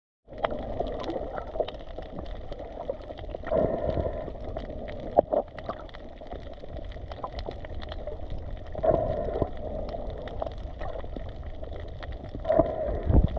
School of redtoothed triggerfish (Odonus niger), Maldives
Big aggregation feeding on zooplankton of redtoothed triggerfish, pejepuerco dentirojo (Odonus niger).
Filmed in Kuramathi Island, Thoddoo Atoll (Alif Alif Atoll, Maldives Islands).